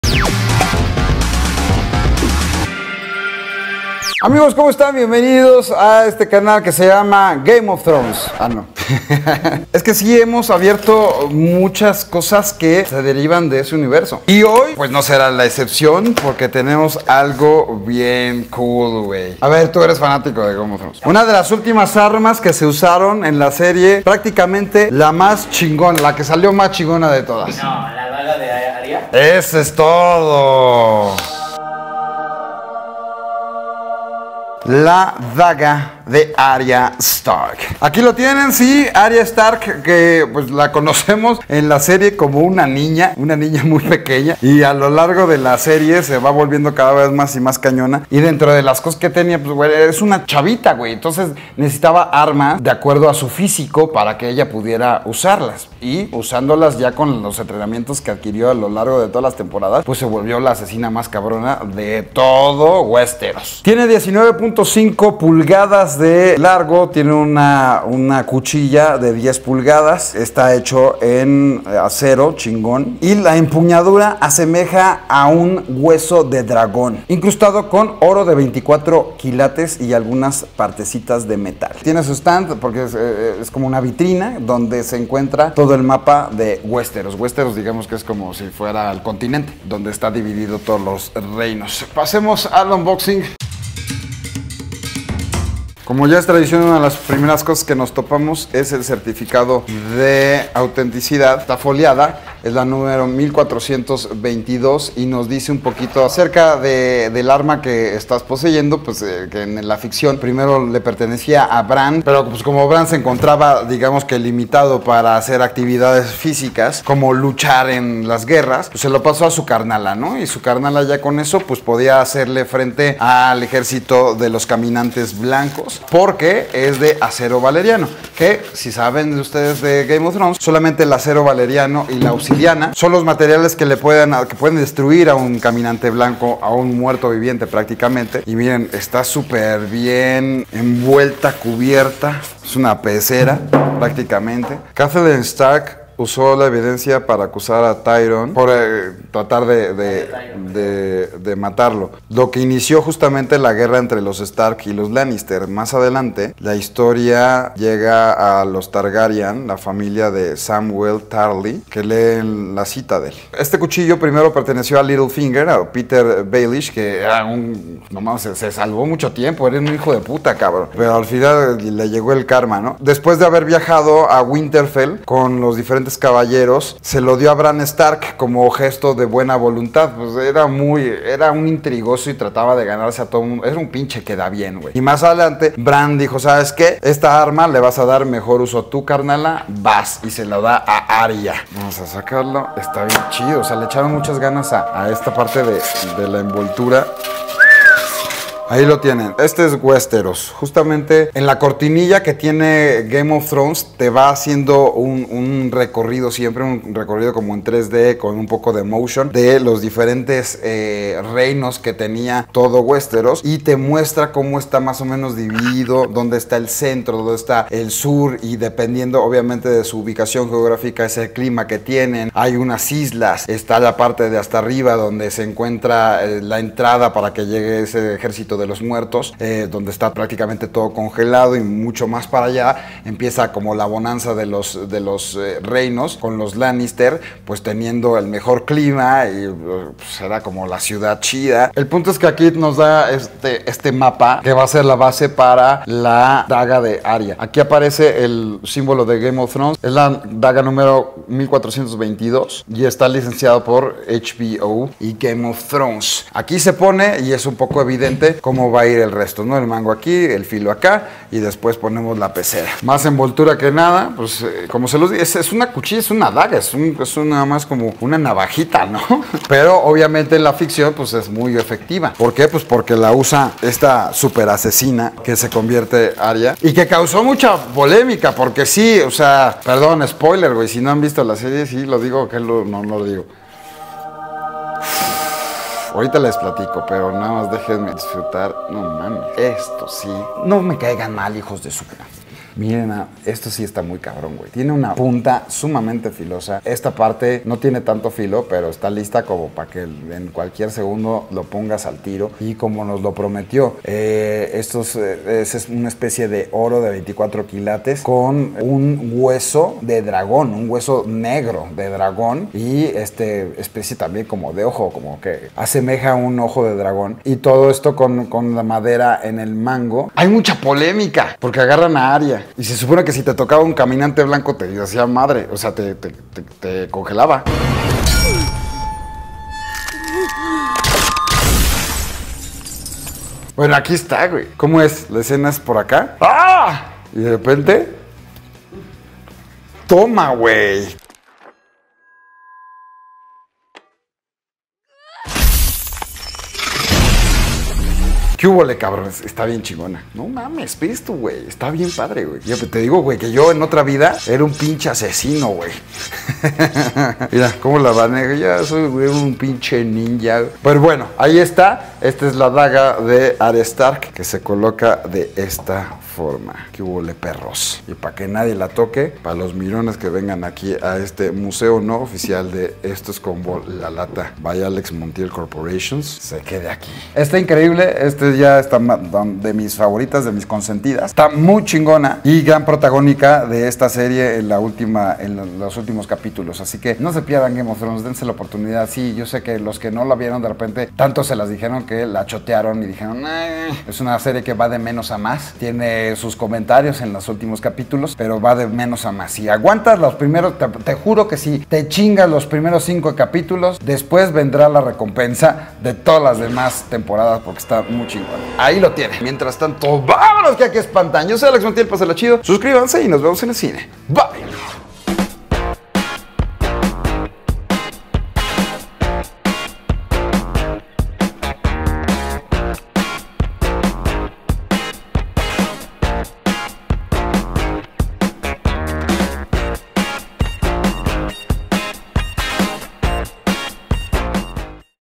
La daga de acero valeriano de ARIA STARK en Game of Thrones
Una daga traída desde Westeros está quí y la comparto con todos ustedes.